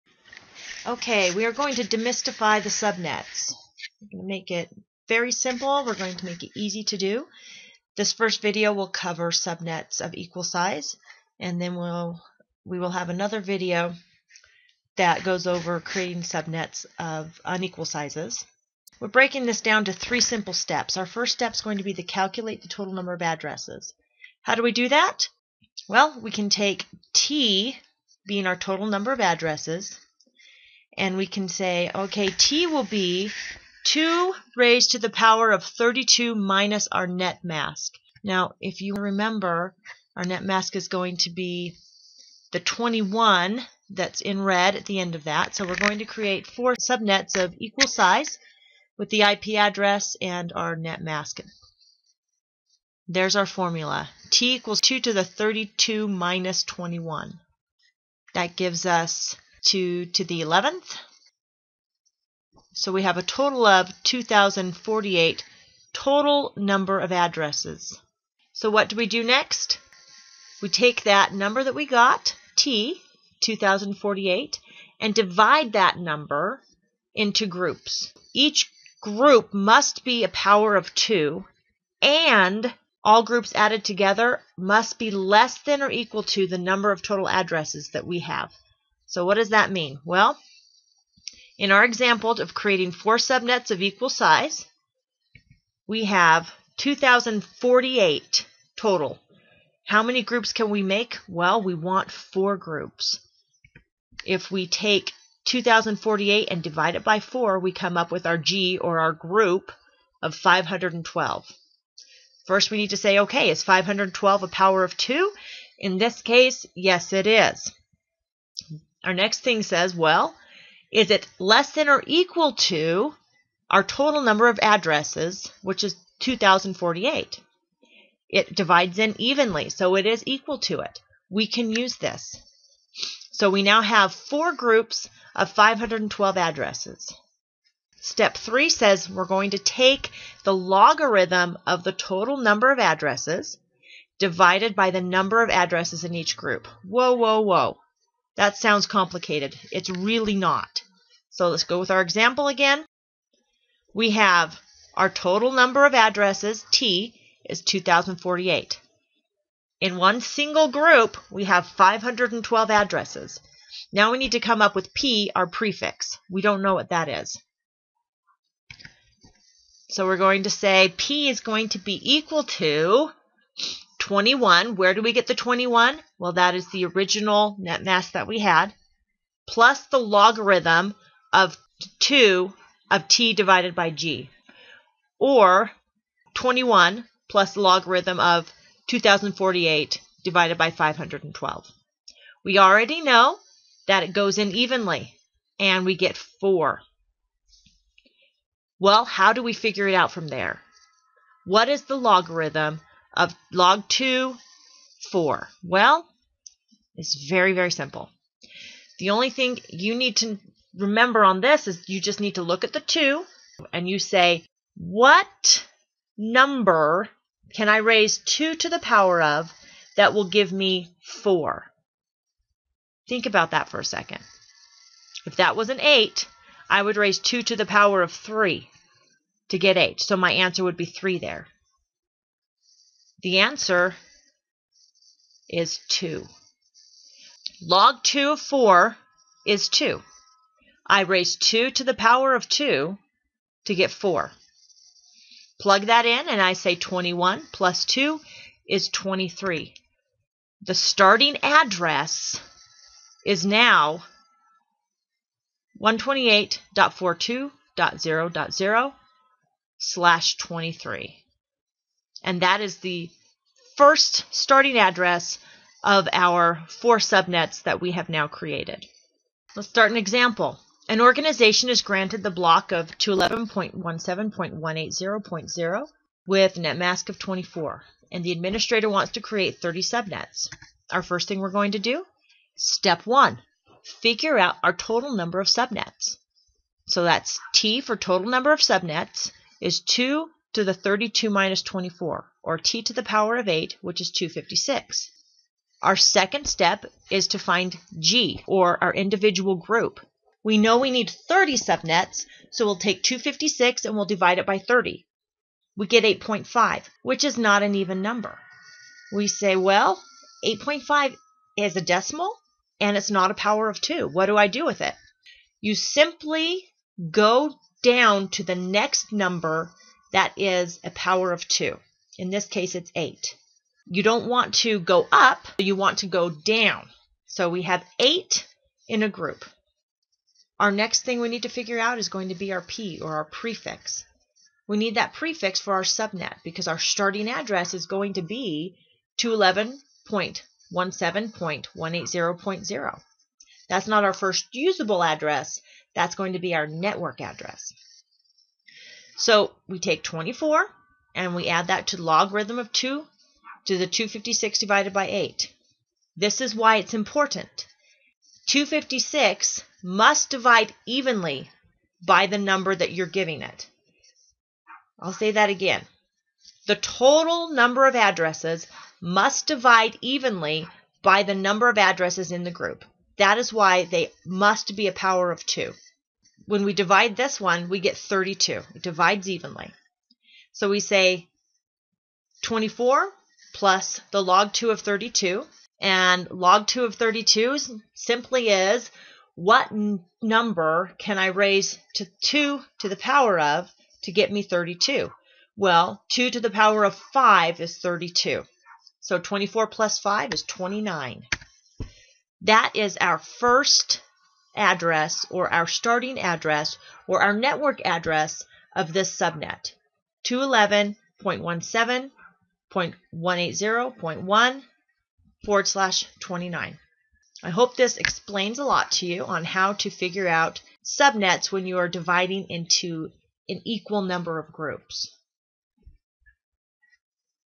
Create Subnets of Equal Size
A short tutorial to create subnets of equal size IP addresses.